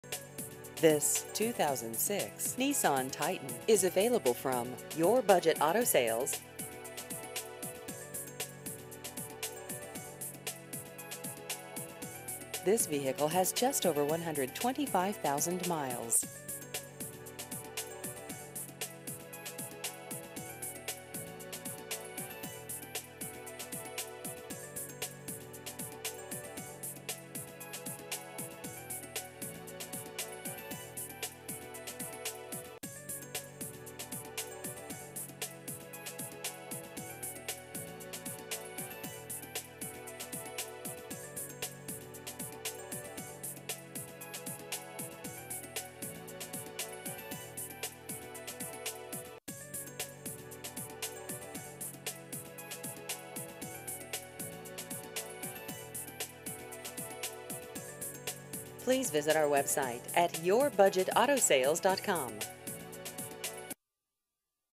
2006 Nissan Titan Used Cars Salt Lake City UT
This 2006 Nissan Titan is available from Your Budget Auto Sales.